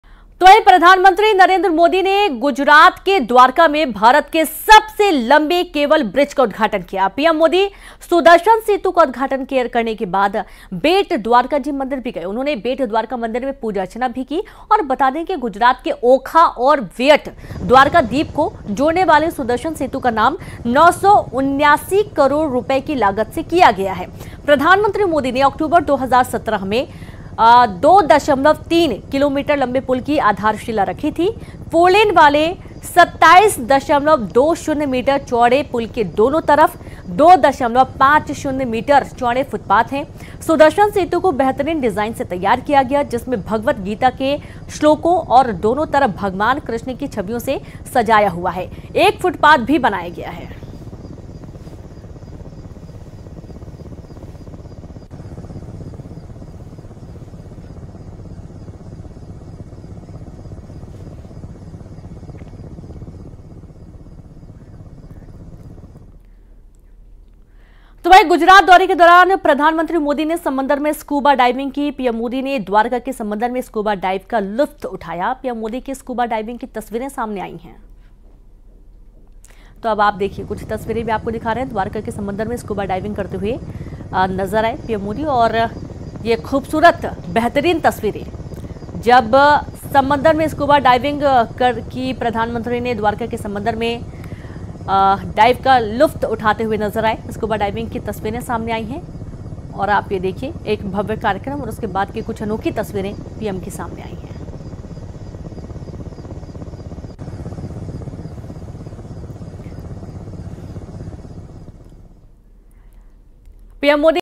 PM Modi Gujarat Visit : देश के सबसे लंबे Cable Bridge Sudarshan Setu का PM Modi ने किया उद्घाटन
PM Modi Gujarat Visit : PM Modi आज Gujarat के दौरे पर हैं. PM Modi ने Gujarat के द्वारका में सबसे लंबी केबल ब्रिज का उद्घाटन किया. Sudarshan Setu का उद्घाटन करने के बाद PM Modi बेट द्वारका जी मंदिर भी गए.